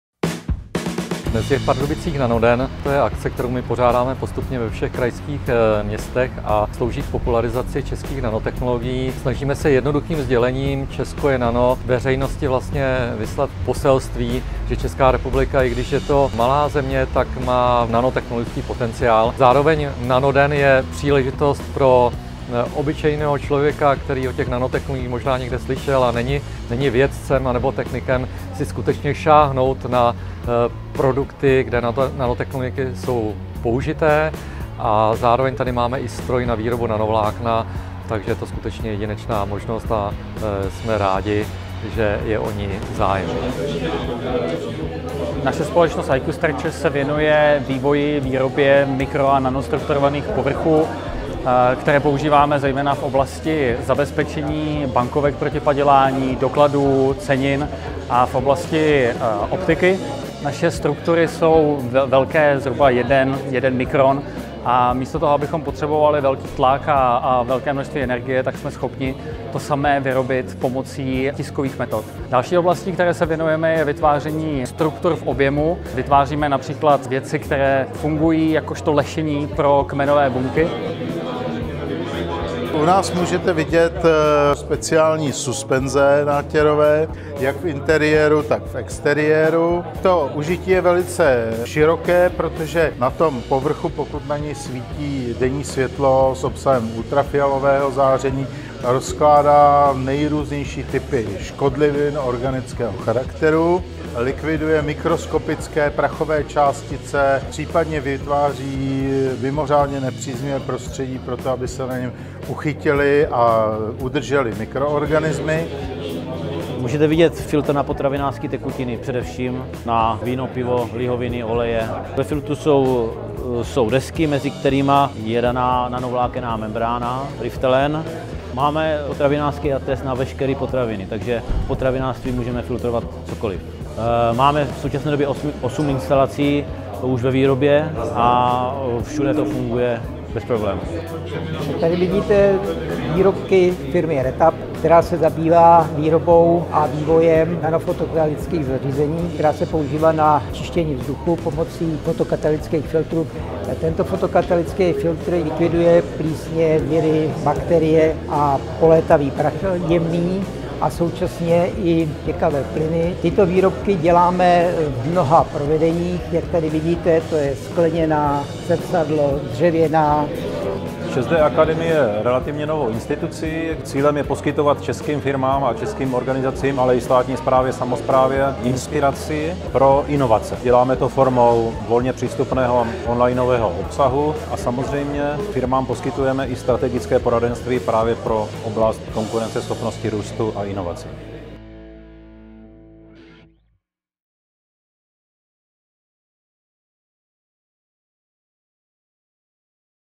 Nanoden Pardubice 2017
Nanoden v Pardubicích: EU vyhlásila soutěž na technologii, kterou už Češi vymysleli

Nanooptika pro automobily, čištění městského vzduchu fotokatalytickými nátěry či nanofiltry na filtraci vína, to vše představili čeští nanotechnologové na Nanodni v Pardubicích, který se uskutečnil v sále Jana Kašpara na Krajském úřadě ve středu 12. dubna.

„Mami, co to je za velikého pavouka? ” Ukazuje na obřího roztoče zvídavě malá holčička. Pardubickému krajskému úřadu dominuje obrovský roztoč. „Chtěli jsme lidem ukázat, kdo nám vlastně způsobuje alergické reakce a před čím chrání protiroztočové lůžkoviny s nanotkaninou,” vysvětluje přítomnost obřího roztoče ředitel nanoSPACE a předseda Asociace nanotechnologického průmyslu ČR Jiří Kůs. Češi jsou v nanovláknu světové jedničky, nanovlákna nebojují jen proti alergii, ale pomáhají i vyhladit vrásky či opravit kosti. Nově by měla ušetřit náklady vinařům.

„Naše nanofiltry nahrazují dnes používané celulózové desky, oproti kterým nezanechávají pachuť a lze je použít opakovaně. Běžně se vyhazuje až 10% produktu, to naše membrány ušetří,” prohlašuje český nanotechnolog Jan Buk z firmy Pardam. Nanofiltry by se měly v budoucnu uplatnit i při filtraci piva.

České nanotechnologie ale nejsou zdaleka jen výrobky z nanovlákna. “Pomoci nanostrukturace povrchu materiálu dokážeme proměnit obyčejnou plastovou destičku ve vysoce kvalitní optiku do domu, kanceláří nebo automobilu. Taková optika je výrazně menší, lehčí, lze ji efektivně vyrábět i v ohromných sériích a umožňuje vznik zcela nových designů a konstrukčních přístupů k výrobě světel “, vysvětluje Martin Jotov z firmy IQ Structures. Taková technologie umožní například výrobu menších, ale mnohem výkonnějších světel u aut nebo třeba plochých brýlí.

„To, že Češi jsou napřed, dokazuje i paradox, že Evropská komise v únoru vyhlásila soutěž zaměřenou na materiály pro čistý vzduch s cílem objevu materiálu, který sníží koncentrace částic v ovzduší ve městech za pomoci inovativních materiálů. Ten už ale existuje,” prohlašuje Kůs. Podle něj jsme si už před dvěma lety při projížďce kolem Barrandova mohli všimnout neobvyklého nápisu na protihlukové bariéře „Tyto plochy čistí vzduch“. Stěna byla natřena fotokatalytickým samočistícím nátěrem s nanočásticemi TiO2.

„Výsledky měření byly více než pozitivní,  jednoznačně dokázaly, že nátěr radikálně snížil jak koncentrace NO, tak i NO2. Kdybychom natřeli fasády v celé Praze, stala by se nejčistějším městem na světě,” komentuje zjištění Pavel Šefl z Advanced Materials-JTJ.

„V oblasti nanotechnologií patří Česká republika k absolutní světové špičce a výsledky těchto úžasných inovací se nám daří převádět do praxe, do konkrétních produktů, které se úspěšně uplatňují na trhu. Nanotechnologie jsou bezpochyby sektorem průmyslu 21. století, my jsme je také zařadili do strategických dokumentů rozvoje vědy a výzkumu a pokud budeme tuto oblast podporovat, máme unikátní možnost stát se v ní lídrem vývoje a zároveň efektivně propojit špičkové vědce s byznysem,“ řekl k návštěvě Nanodne v Pardubicích vicepremiér pro vědu, výzkum a inovace Pavel Bělobrádek.

Nanoden uspořádala Asociace nanotechnologického průmyslu ČR v rámci edukativního projektu na podporu českých nanotechnologií Česko je nano.